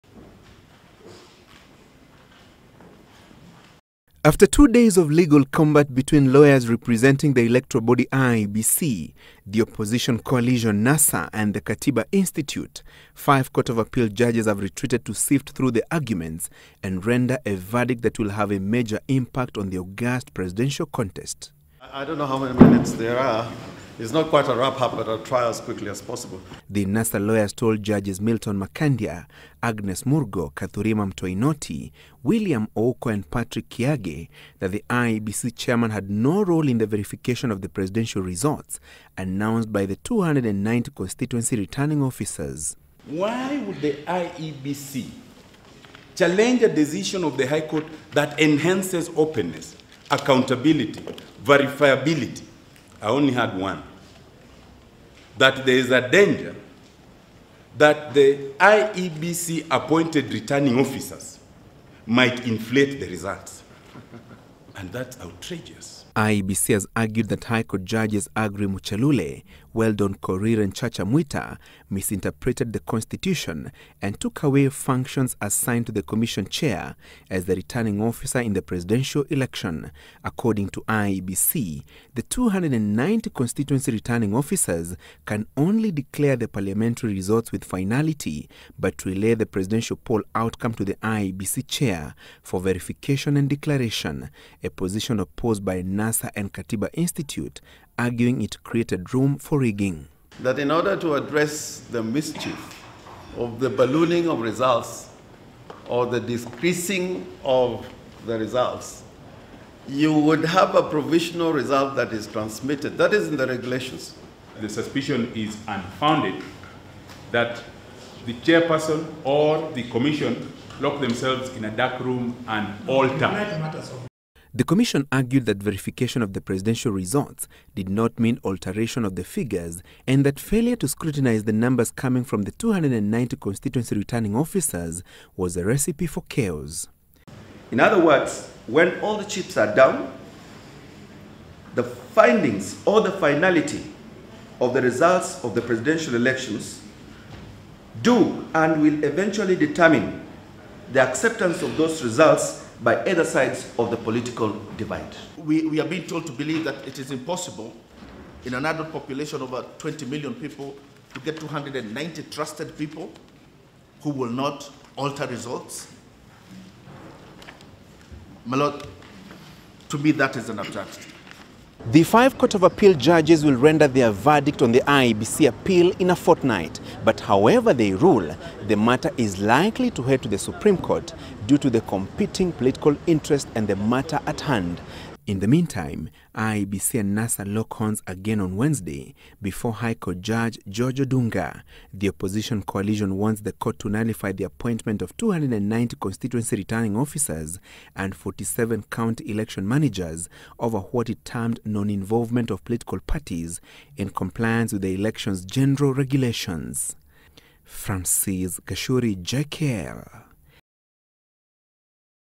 Appeal Court concludes hearing IEBC appeal
The court of appeal will on the 23rd of this month rule whether presidential election results announced in constituencies will be final or are subject to verification at IEBC headquarters.
This follows the conclusion of the hearing of an appeal in which the electoral commission wants powers to declare presidential results reserved for Chairman Wafula Chebukati.